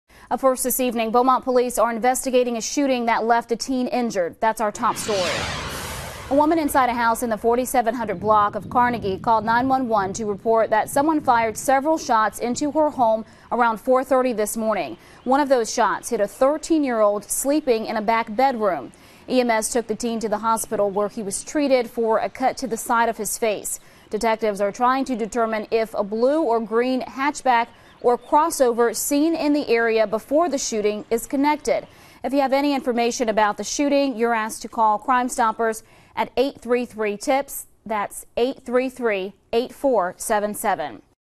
13-year-old hurt after several shots fired at Beaumont home
A woman called 9-1-1 to report that someone had fired several shots into her home around 4:30 this morning in the 4700 block of Carnegie

One of those shots hits a 13 year old sleeping in a back bedroom. EMS took the teen to the hospital where he was treated for a cut to the side of his face.

Detectives are trying to determine if a blue or green hatchback or crossover seen in the area before the shooting is connected.

If you have any information about the shooting, you're asked to call Crime Stoppers at 833-TIPS.